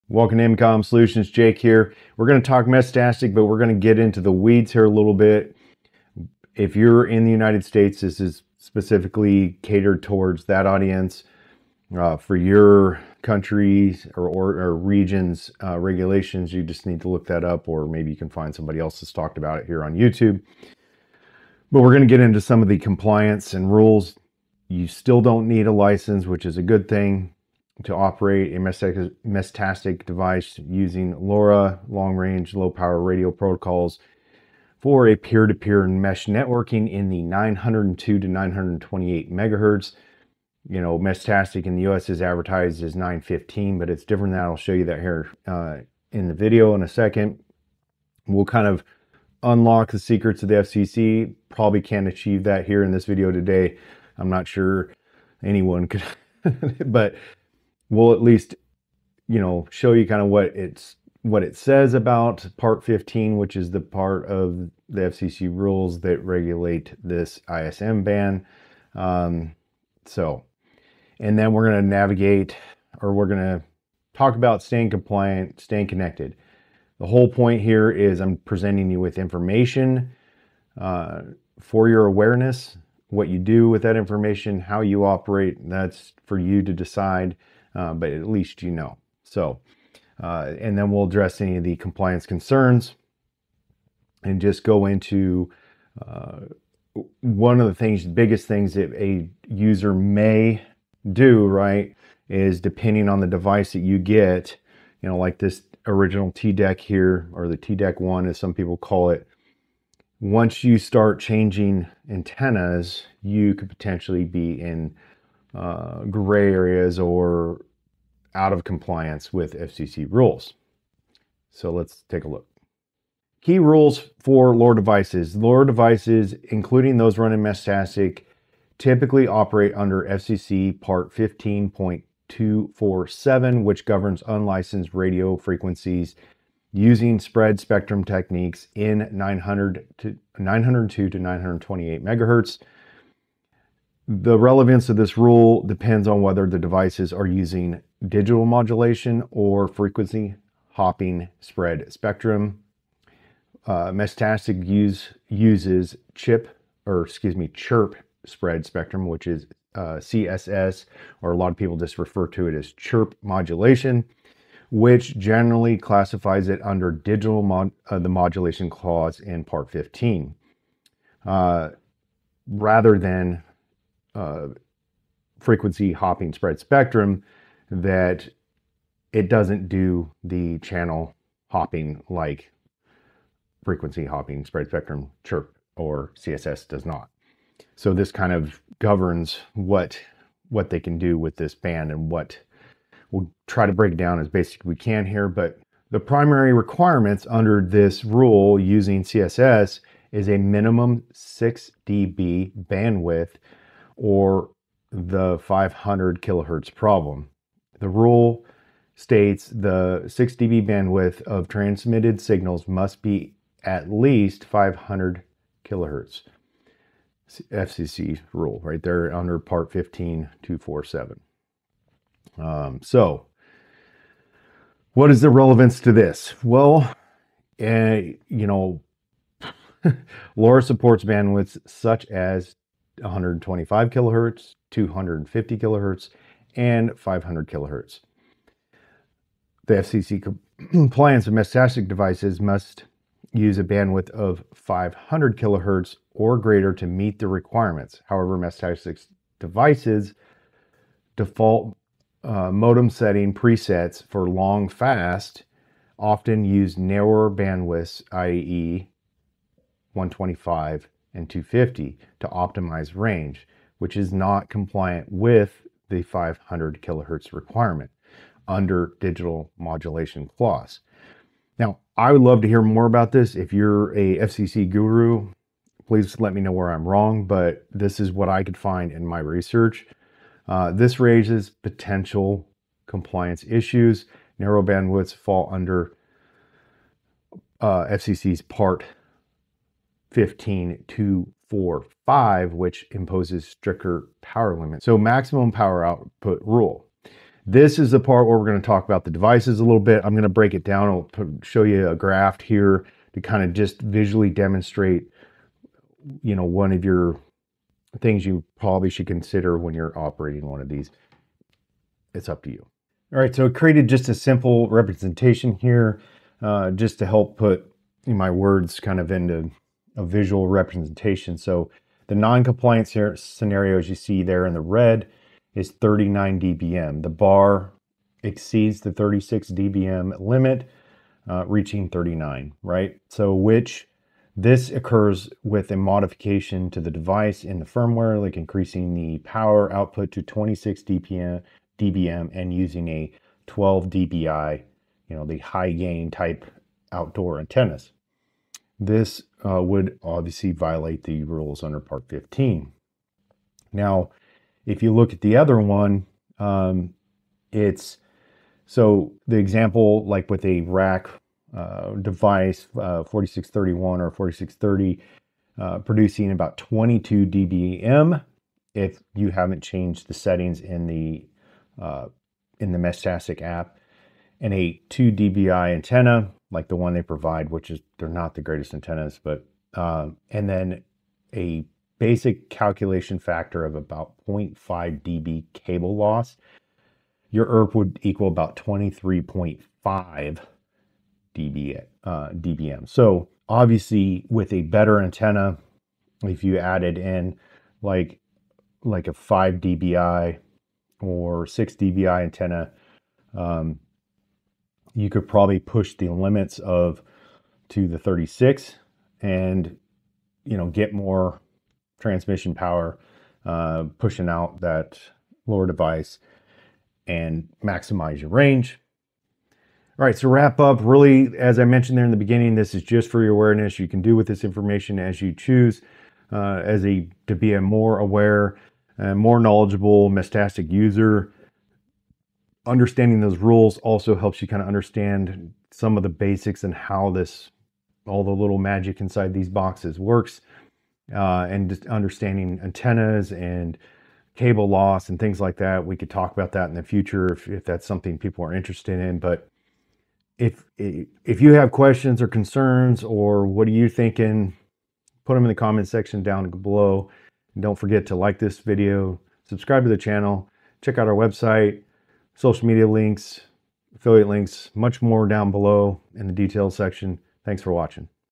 Meshtastic & FCC Part 15 Rules
Today's video covers the FCC's Part 15 rules that apply to Meshtastic/LoRa Radios. The key point of this video is make a more informed Meshtastic user.

How to calculate EIRP: EIRP (dBm) = Transmitter Power (dBm) + Antenna Gain (dBi) - Cable Loss (dB)